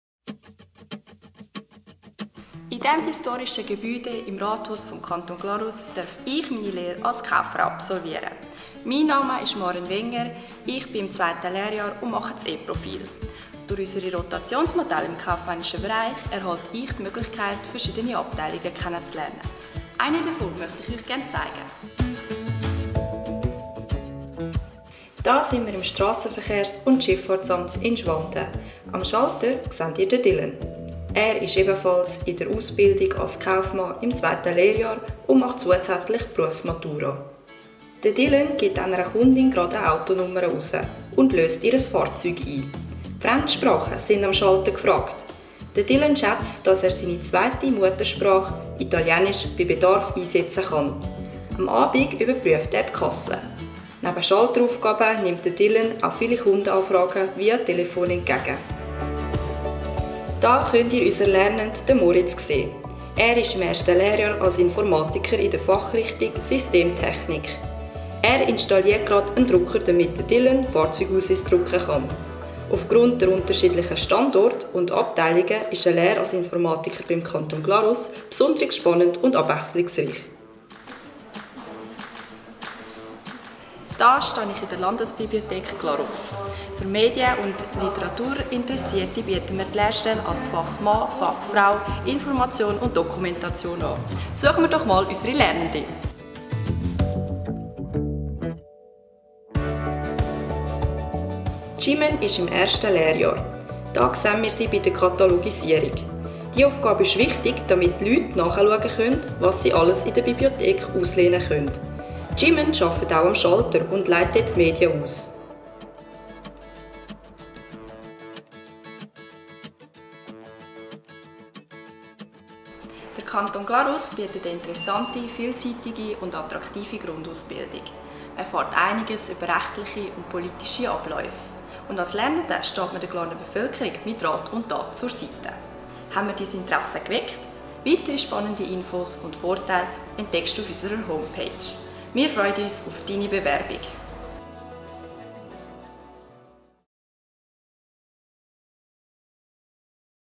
Deine Lehre beim Kanton Glarus
Der Kanton Glarus ist ein moderner und attraktiver Lehrbetrieb und bietet über 20 spannende Ausbildungsplätze in verschiedenen Abteilungen.